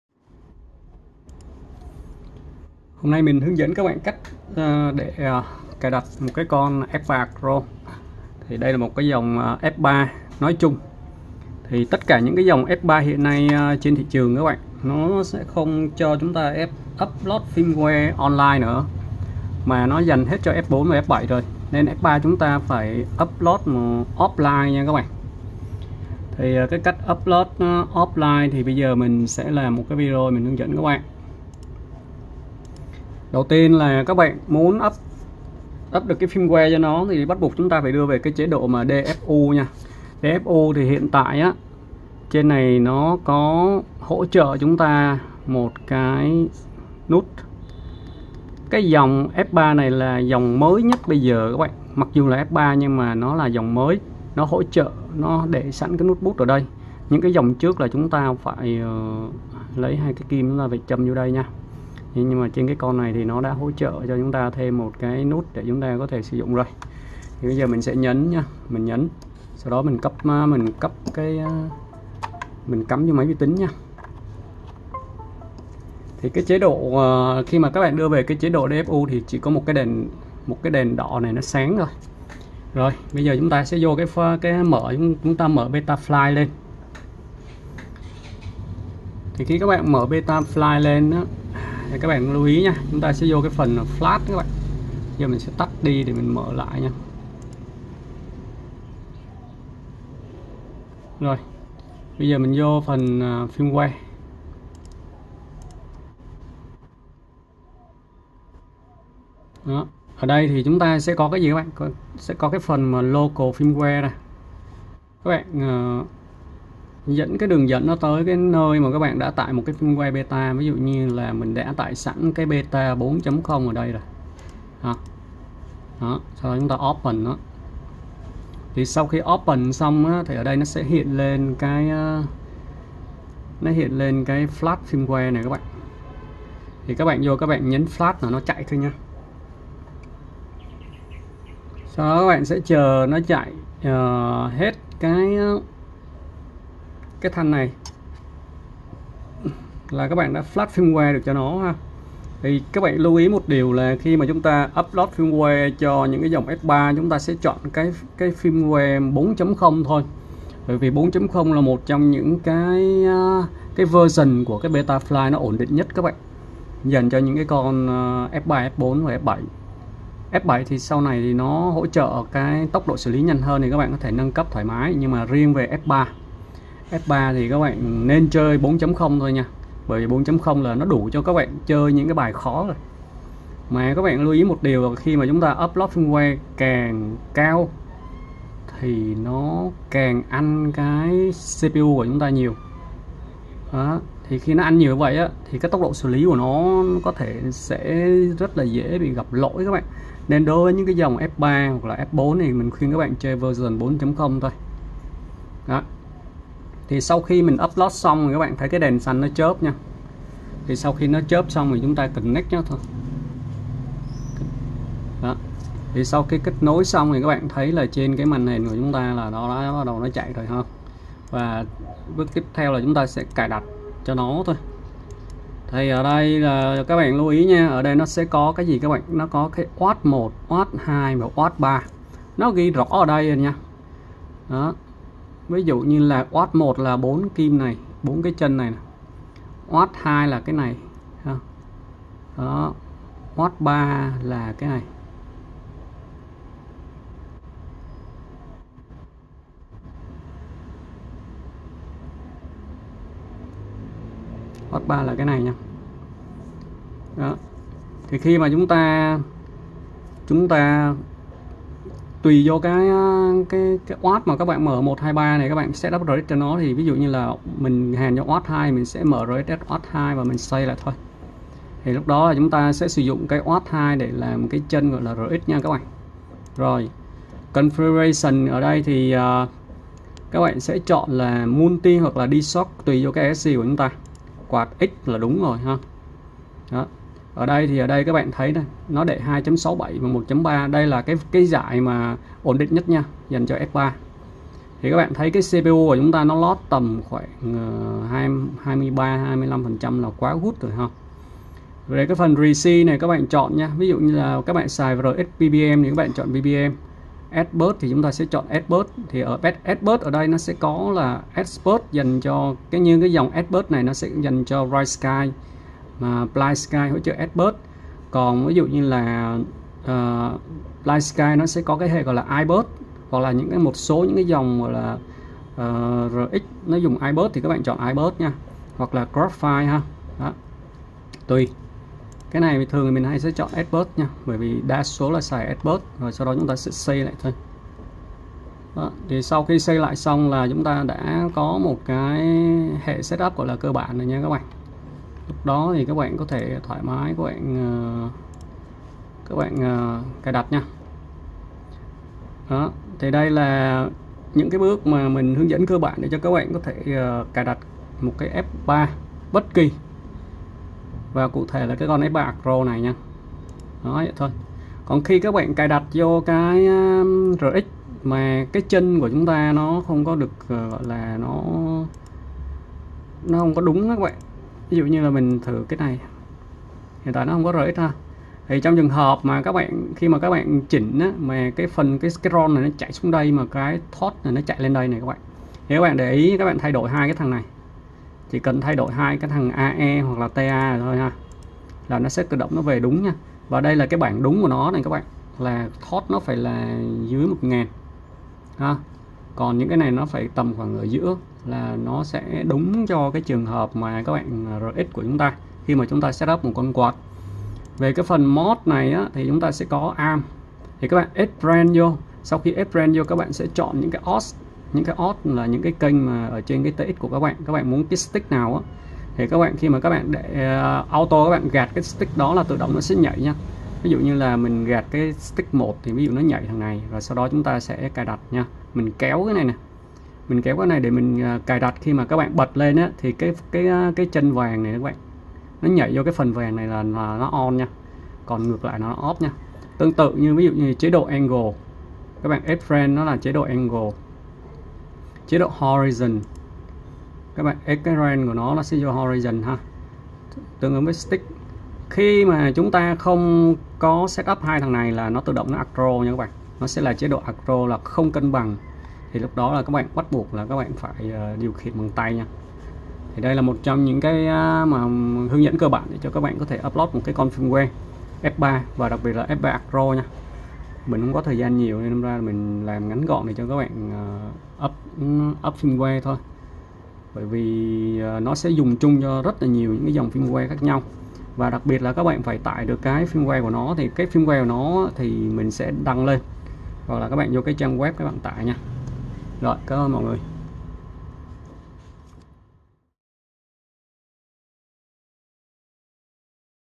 Nâng cấp cài đặt firmware offline cho các dòng mạch F3 hiện nay
hướng dẫn anh em newbie cài đặt nâng cấp fw cho các dòng f3 hiện nay ko hỗ trợ trên server của betaflight